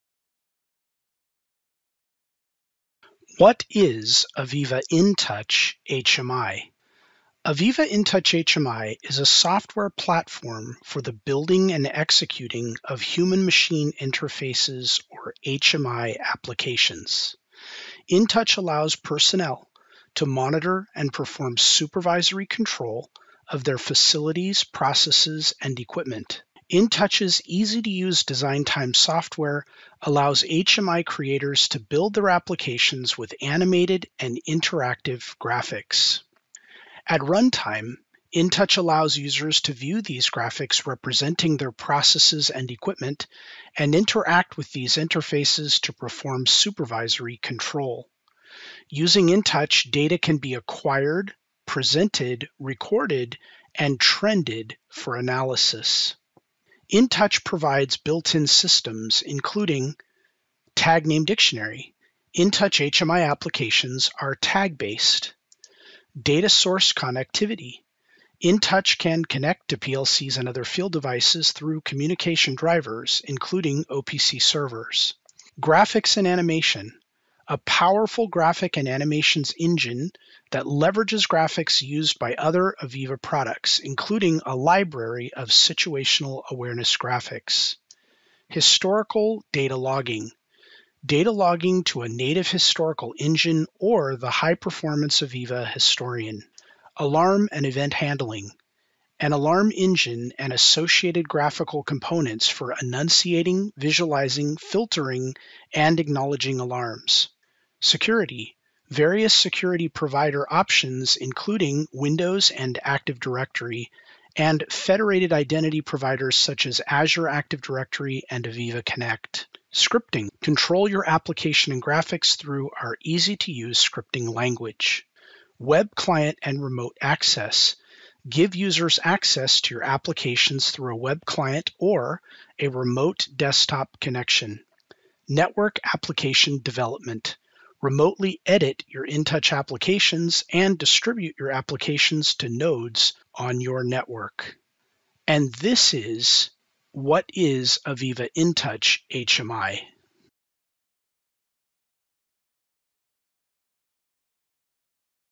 What is AVEVA™ InTouch HMI?
What is AVEVA™ InTouch HMI? This video explains the features and functionalities of AVEVA™ InTouch HMI, including its uses for building and executing HMI applications, monitoring and controlling facilities, and analyzing data. 🧑‍💻